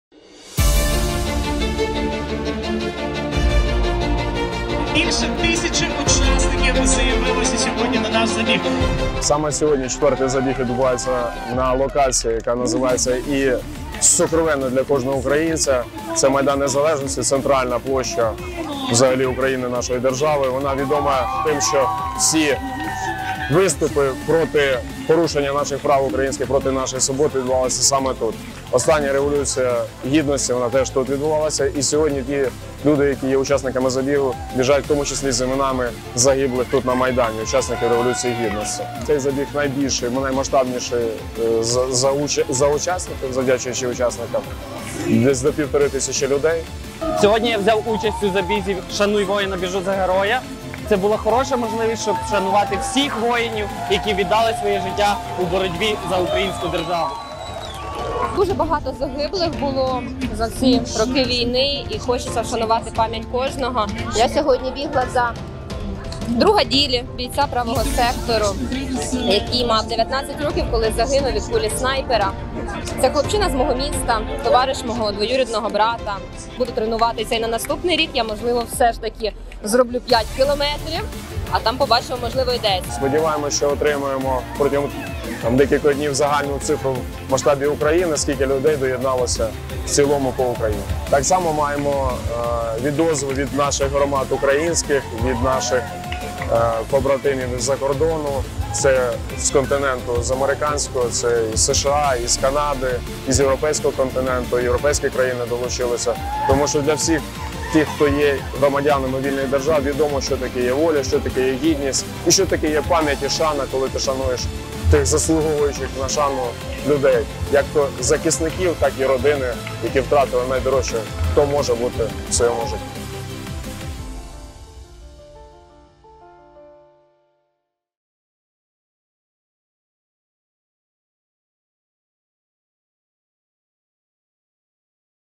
Шануємо воїнів, біжимо за Героїв України - 2021 | ОпірTV
В 26 містах і за кордоном пройшов забіг на честь загиблих воїнів

28 серпня патріоти вшанували пам'ять загиблих українських воїнів на Всеукраїнському забігу "Шаную воїнів, біжу за Героїв України".
В самому лише Києві захід зібрав понад тисячу учасників. Символічно, що забіг стартував саме на Майдані Незалежності. Учасники вибирали або отримували за розподілом ім'я героя, на честь якого біг 2, 5 або 10 кілометрів.

Так громадяни засвідчили свою вдячність тим, хто загинув за вільну Україну і показали, що пам'ять про воїнів жива і для нас вони досі в строю.

Забіг пройшов у 26 містах України, а також до нього долучилася діаспора в США та Канаді.